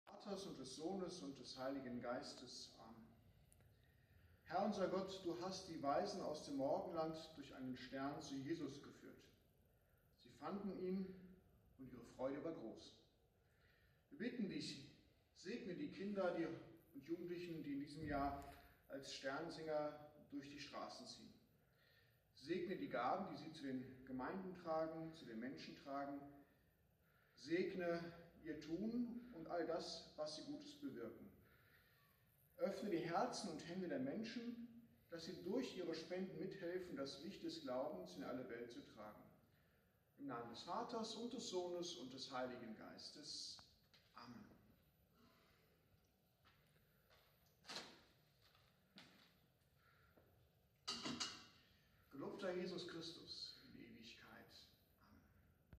Die Segenstüten der Sternsinger Schwelm sind bereit
Propst Norbert Dudek hat die Segenstüten, die die Sternsinger Schwelm dieses Jahr coronakonform an alle Spender verteilen, gesegnet. Es kann also losgehen. Nach dem Motto: GemeinsamGehts. Lasst uns die Welt verändern – kreativ und vielfältig.